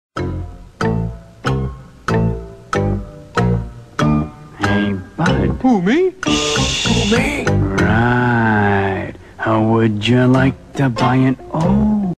Hey Bud! C'mere...
Ernie receives a fortunate proposal from  a friendly stranger.